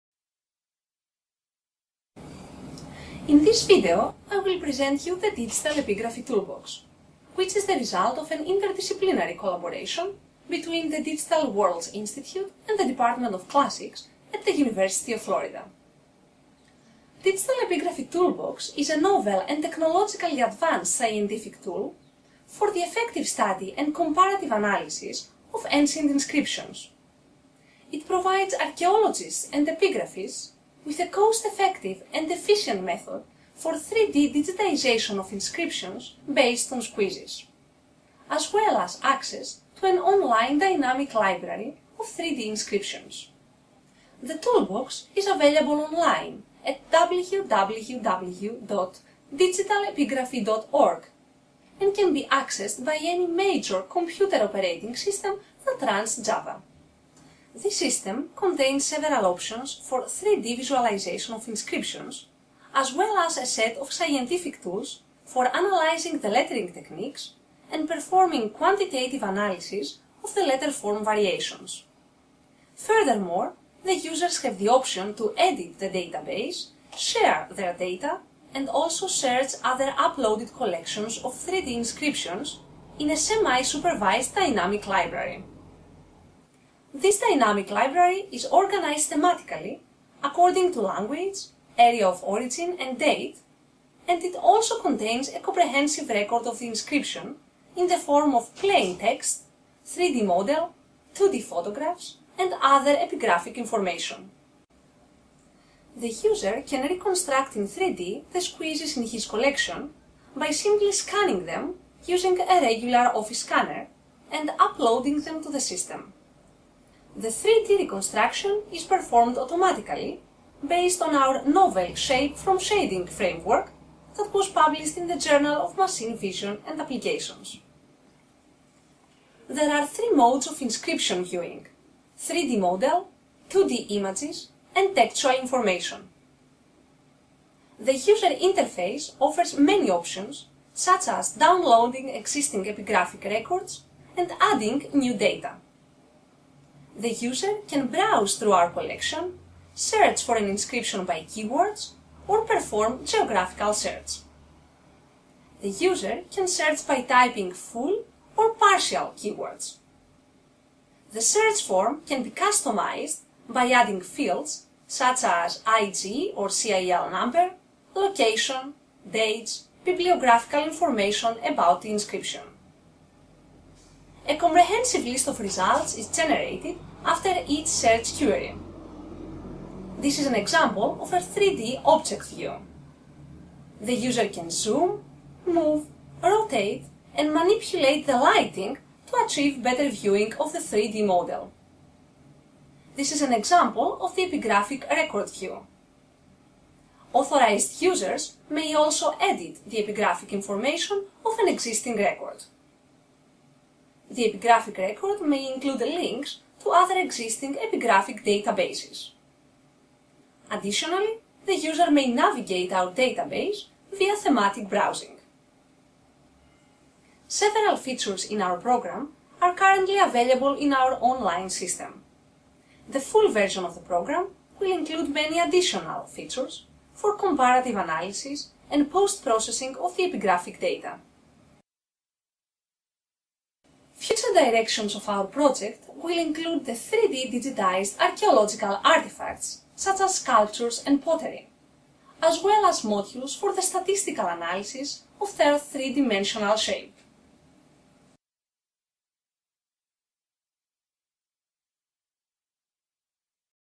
Digital Epigraphy Toolbox
The goal of this presentation is to introduce the Digital Epigraphy Toolbox, a novel and technologically advanced scientific tool for the effective study and comparative analysis of Greek and Latin inscriptions. This toolbox provides archaeologists and epigraphists with a cost-effective and efficient method for 3D digitization of inscriptions based on squeezes, as well as access to an online dynamic library of 3D inscriptions.

More specifically, the Digital Epigraphy Toolbox is a publicly available web-application designed to facilitate the digital preservation and electronic dissemination of both Greek and Latin inscriptions. It allows epigraphists to digitize in 3D their squeezes, while it overcomes the limitations of  current methods for digitizing epigraphic data in 2 dimensions only.  This system contains several options for 3D visualization of inscriptions, as well as a set of scientific tools for analyzing the lettering techniques and performing quantitative analysis of the letterform variations. Furthermore, the users have the option to share their data and also search  other uploaded collections of 3D inscriptions in a semi-supervised dynamic library. This dynamic library is organized thematically according to language, area of origin, and date and it also contains a comprehensive record of the inscription in the form of plain text, 3D model, 2D photographs, and other epigraphic information.

There are 3 modes of inscription viewing: 3D  model, 2D images, and textual information. The user interface offers many options, such as downloading existing epigraphic records and adding new data. The user can browse through our collection, search for an inscription by keywords, or perform geographical search. The user can search by typing full or partial keywords. The search form can be customized by adding fields, such as IG/CIL number, location, dates, bibliographical information about an inscription.

A comprehensive list of results is generated after each search query. The user can zoom, move, rotate, and manipulate the lighting to achieve better viewing of the 3D model. Authorized users may edit the epigraphic information of an existing record. The epigraphic record may include links to other existing epigraphic databases. Additionally, the user may navigate our database via thematic browsing.

Digital Epigraphy Toolbox was funded in part by the NEH grant HD-51214-11.